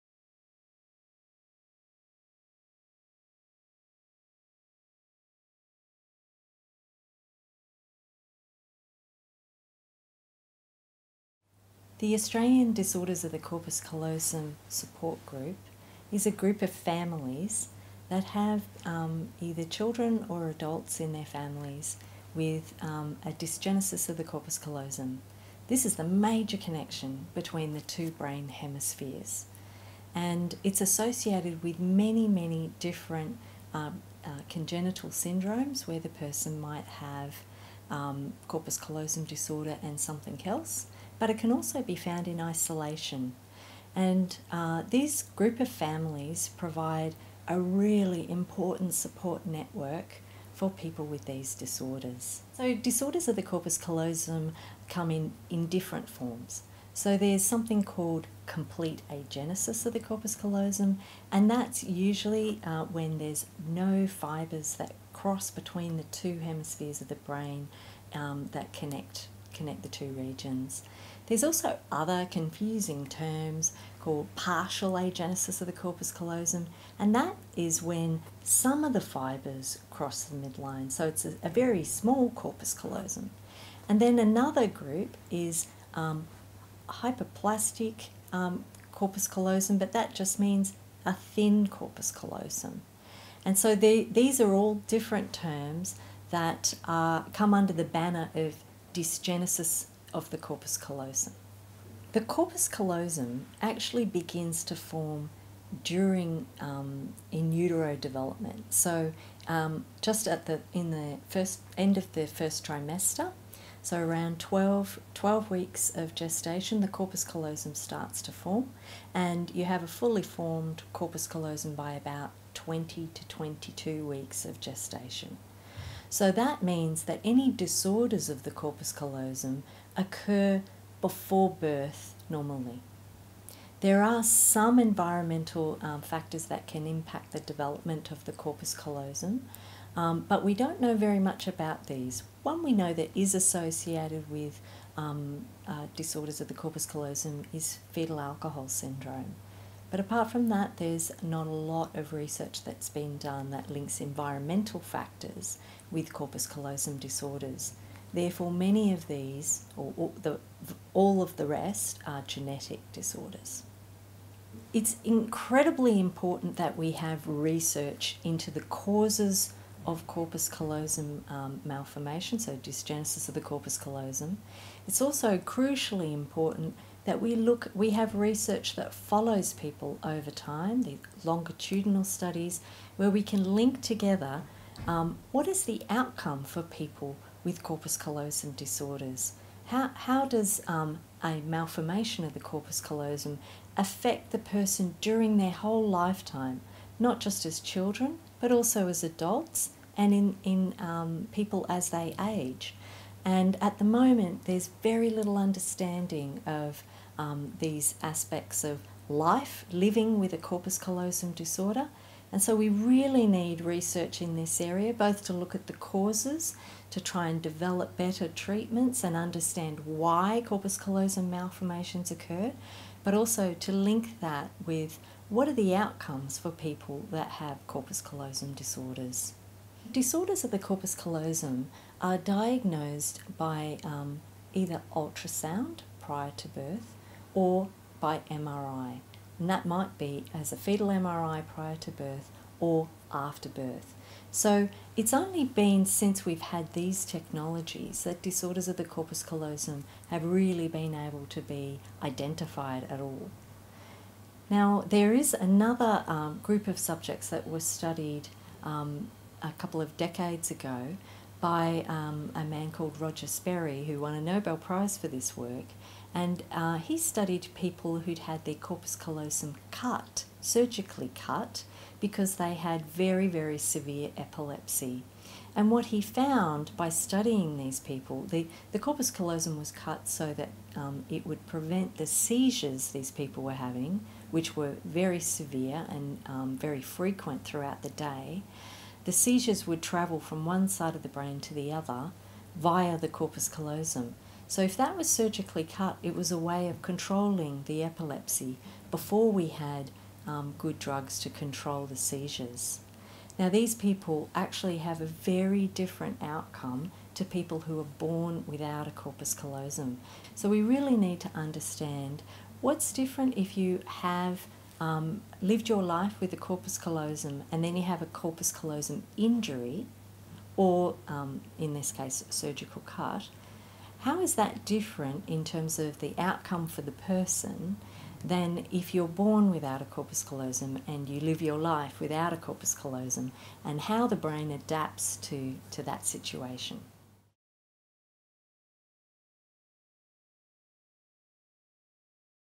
#6 The Corpus Callosum with Professor Linda Richards
Professor Linda Richards is Australia's leading scientist researching  disorders of the corpus callosum (DCC). She leads a team of scientists at Queensland Brain Institute (QBI). They are using state of the art research techniques to learn more about the corpus callosum and other brain areas.

The corpus callosum is the major connecting tissue joining the 2 halves of the brain. Some people are born missing part of all of their corpus callosum.

Professor Richards is AusDoCC's Chief Scientific Advisor and patron.
Contact AusDoCC, Australia's national support group, to learn more about corpus callosum disorders and support available in Australia.

Hear Professor Richards speak about the corpus callosum and AusDoCC.